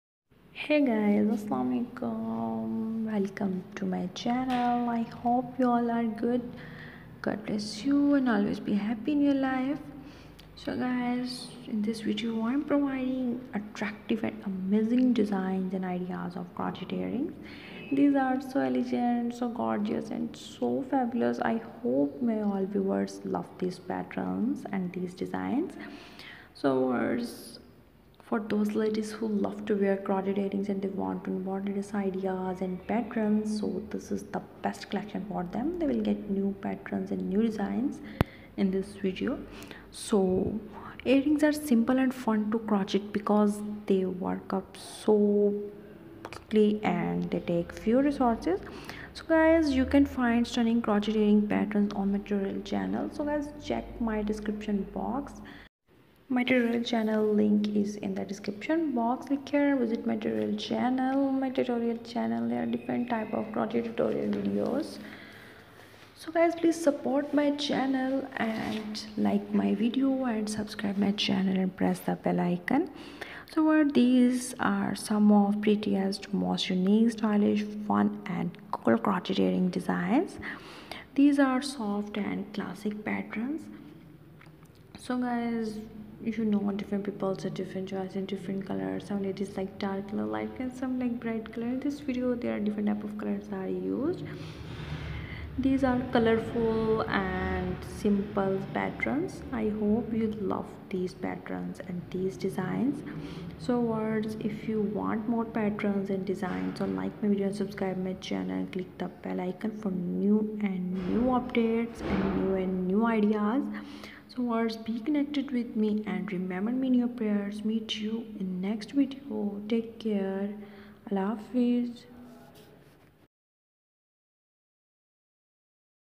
New Classy Hand knitted CROCHET Earrings for Girls--New Trendy Patterns
You may think that free crochet earring patterns aren't that special but we would disagree. That's why we put together this page complete with 32 Elegant Crochet Earring Patterns That Will Give You Goosebumps.

These are some of the prettiest, most unique, stylish, fun, and cool crochet earring patterns ever, and they are free to boot!
We promise that you will find at least one pair you'll want to start crocheting right away. They are separated by categories to make it easier to locate the style you desire. First up are the most beautiful vintage style earring patterns.

Crochet earrings can be a bit tedious because they are small and use crochet thread, however, those aspects are also part of their strengths. Because they are small, they work up quickly. You also get a lot of pretty detail when using the thin crochet thread vs yarn. So, take a look and be prepared for some chills!

My Tutorial Channel "Coste Crocheting"